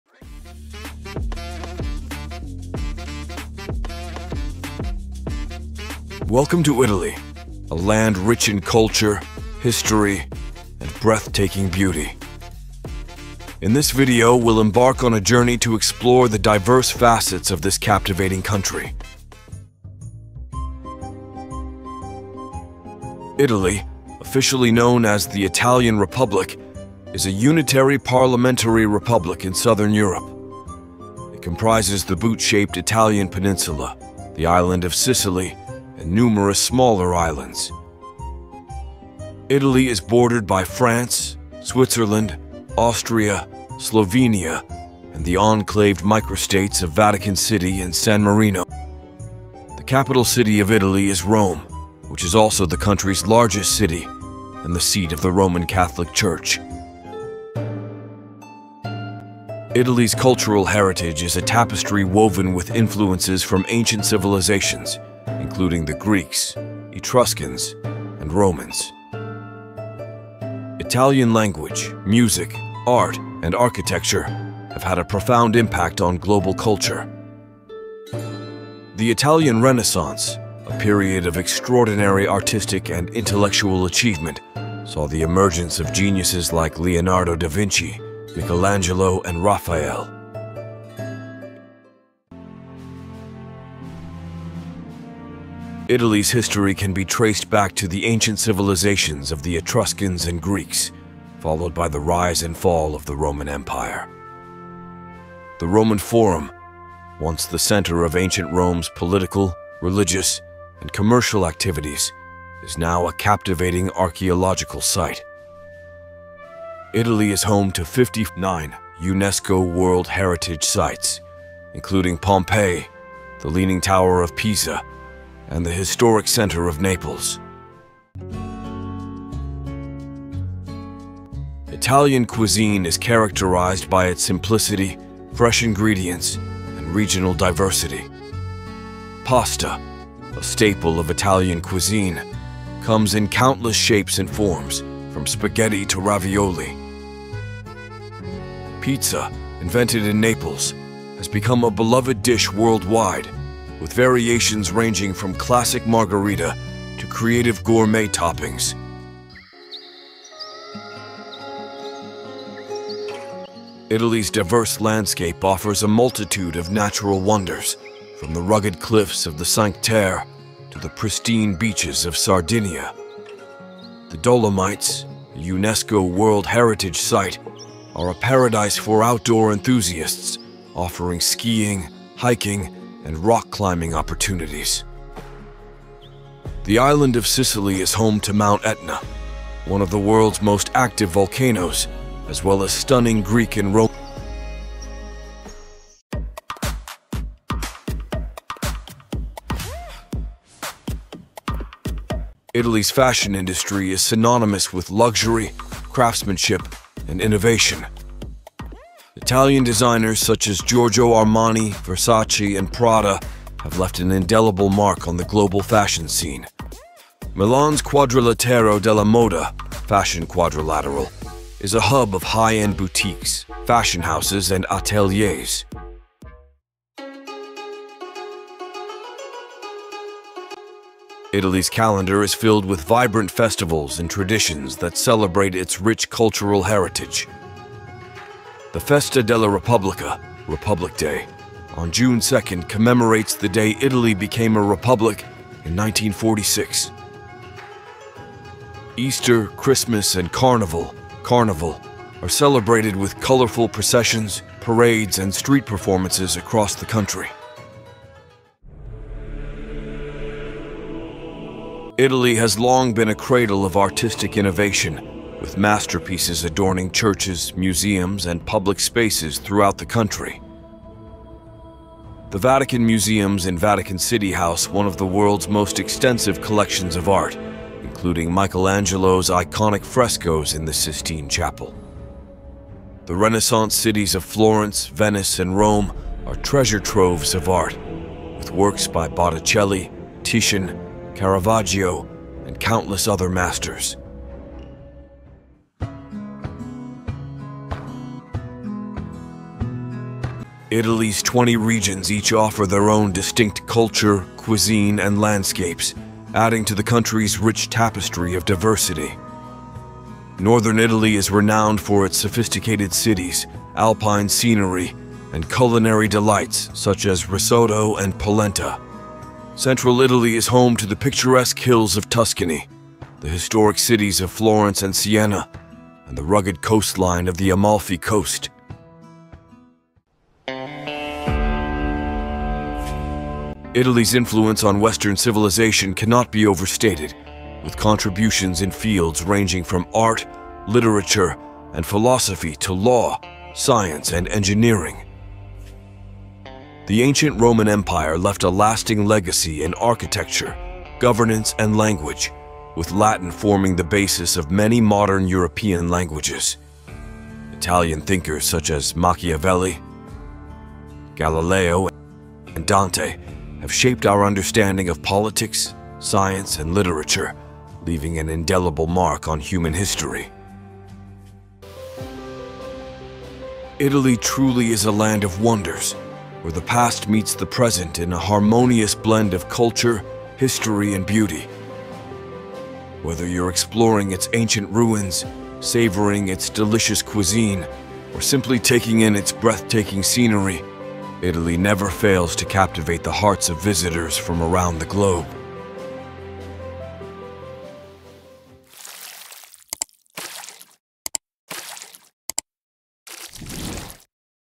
Discovering Italy: A Journey Through Culture, History, and Beauty
🇮🇹 Explore Italy: A Journey Through Culture, History, and Beauty 🇮🇹

Join us on an immersive journey through Italy, where we'll uncover the rich tapestry of this captivating country's culture, history, and natural beauty. From ancient Roman ruins to Renaissance art and modern fashion, Italy offers a feast for the senses and a glimpse into its vibrant heritage.

📌 Video Information:
🎥 Title: Discovering Italy: A Journey Through Culture, History, and Beauty
🕒 Duration: 6:45 minute

🔹 Timeline:
0:00​ - Introduction to Italy
0:5​7 - Cultural Heritage
1:32​ - History and Landmarks
2:09 - Culinary Delights
2:40​ - Natural Beauty
3:18​ - Fashion and Design
3:46​ - Festivals and Traditions
4:19​ - Artistic Legacy
4:56​ - Regional Diversity
5:30​ - Influence on Western Civilization
6:10​ - Conclusion

Don't miss this opportunity to embark on a virtual tour of Italy and discover the wonders that await in this beautiful country.